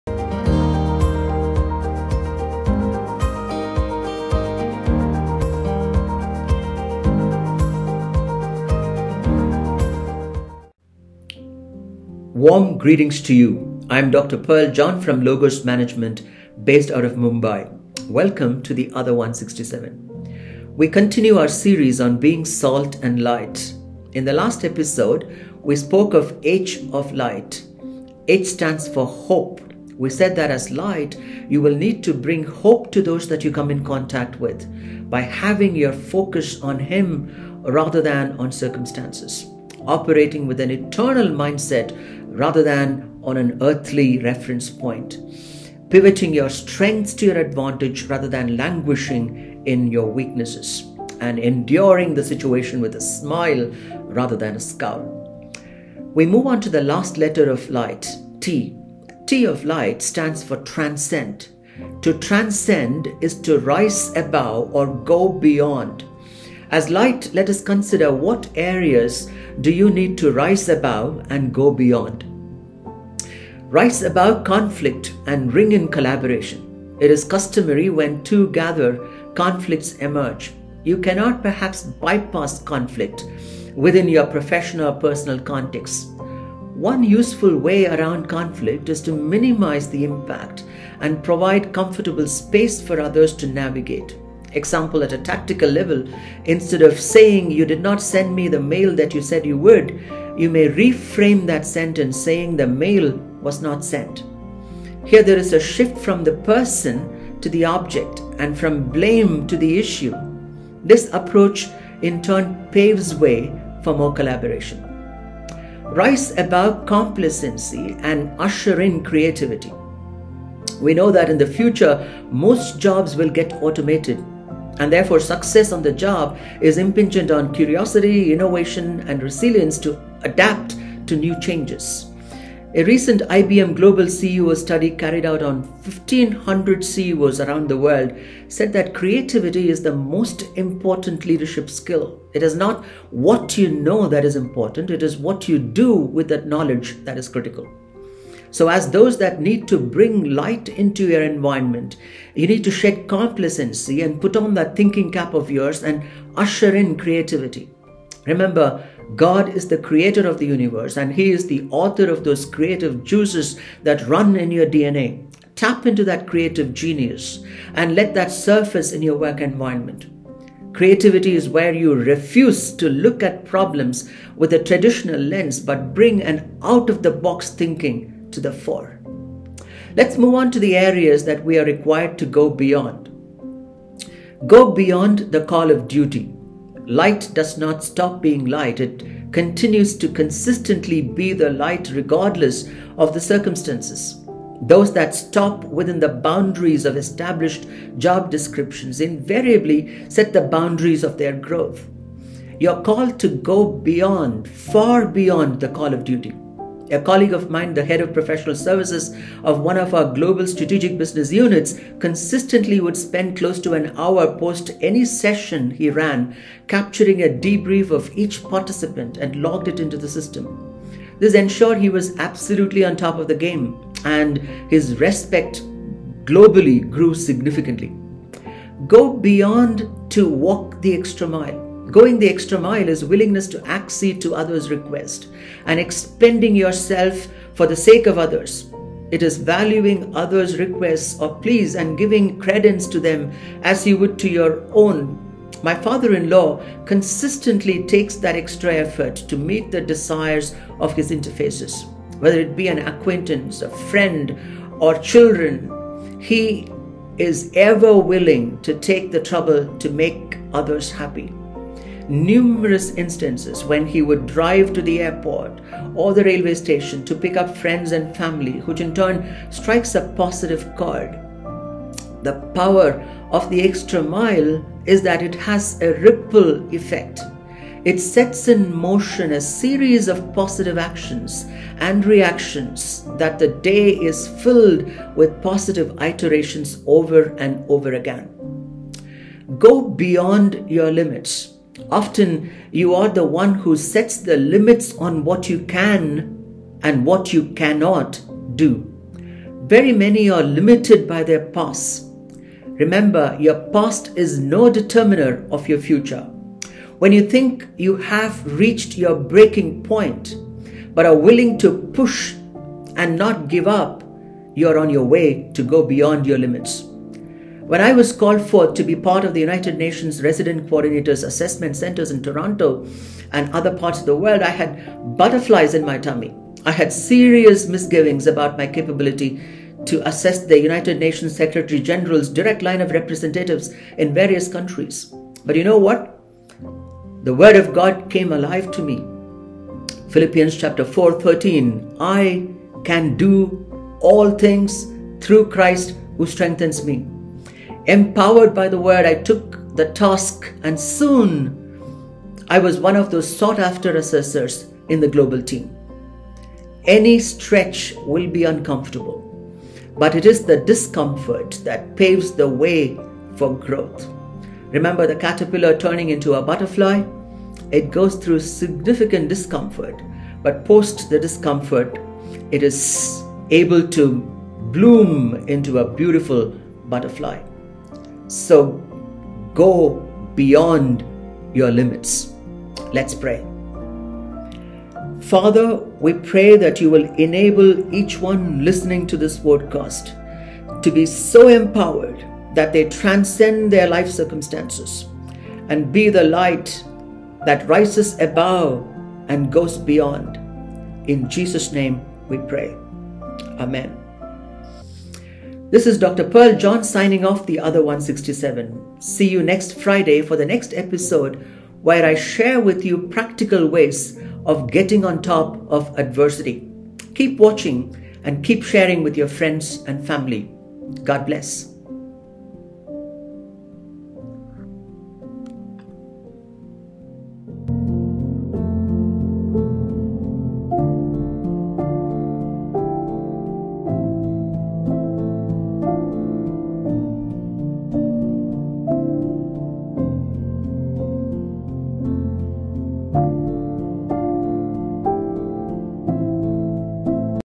Salt & Light - Part 10 | Dr. Pearl John | The Other 167
DAY 481
November 26 , 2021
Friday

Title :-   Salt & Light - Part 10

Philippians 4:13

👉  Rise above conflict and ring in collaboration
👉  Rise above complacency and usher in creativity
👉  Go beyond the call of duty
👉   Go beyond to walk the extra mile
👉  Go beyond your limits

Let's tune in to Dr. Pearl John - Executive Director - Logos Management -  India  to learn more about  Salt & Light - Part 10

Godspeed!!